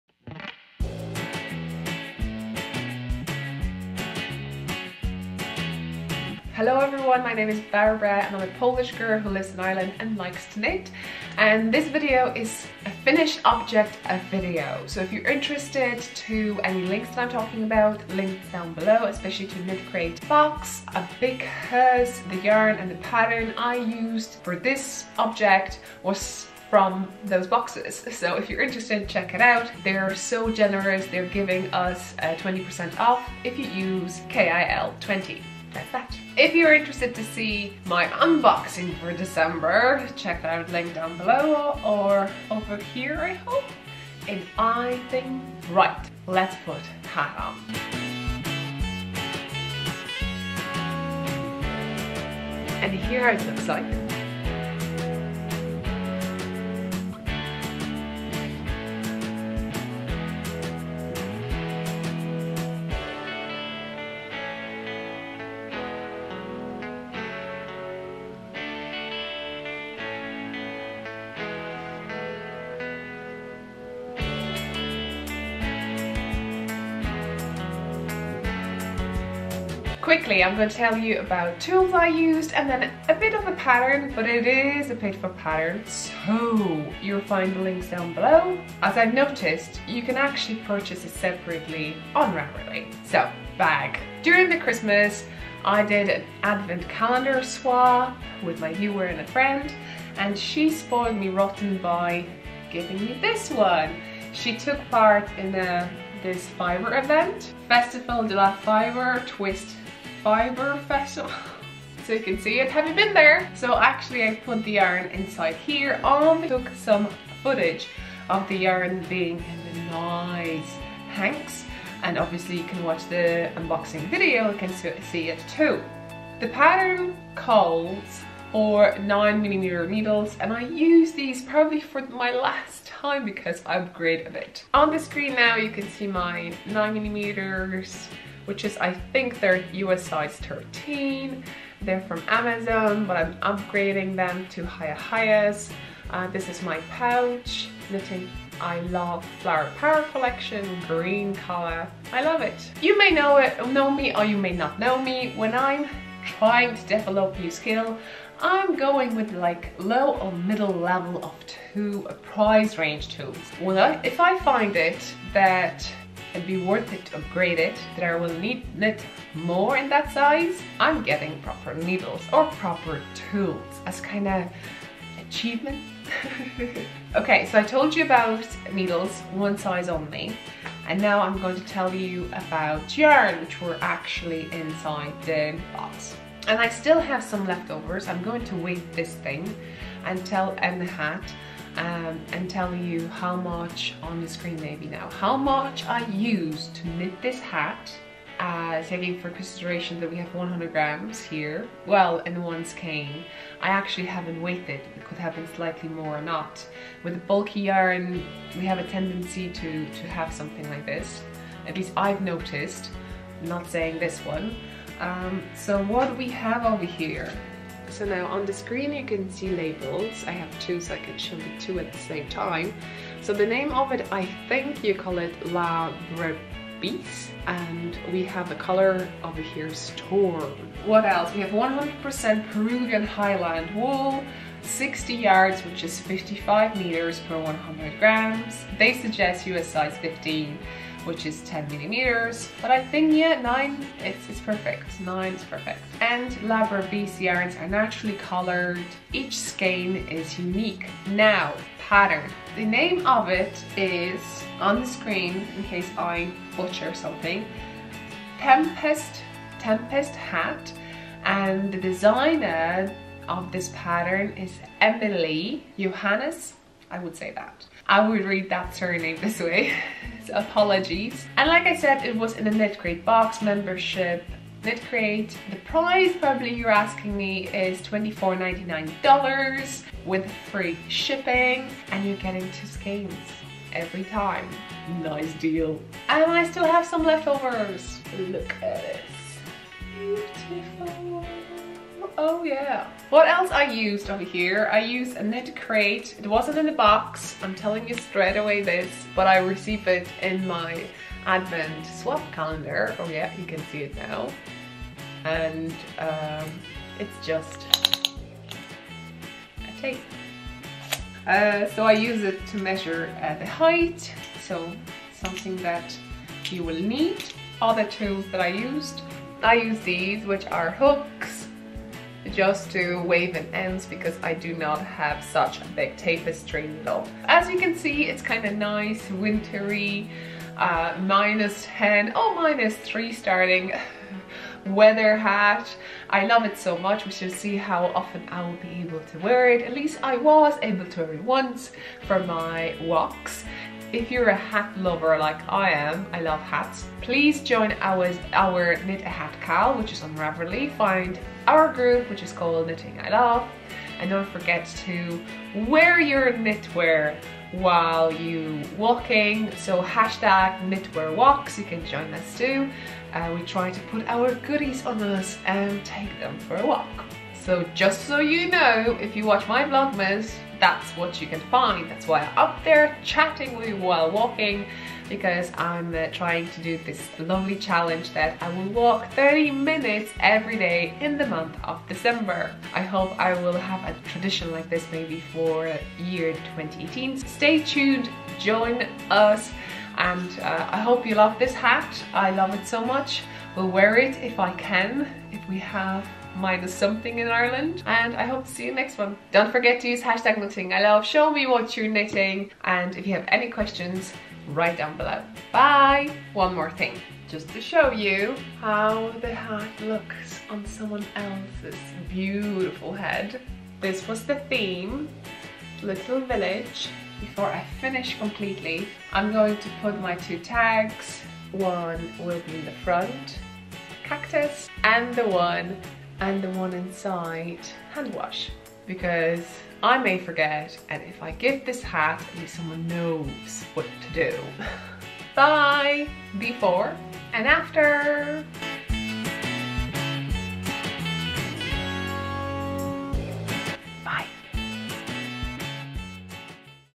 Tempest HAT by Emily Johannes from KnitCrate box ♥︎ Finished Object ♥︎ knitting ILove
I love hats and if I see a hat to knit with a bulky yarn pretty much I would not say no to knit it as it is a quick knit and such a fun knitwear to wear. Thank you KnitCrate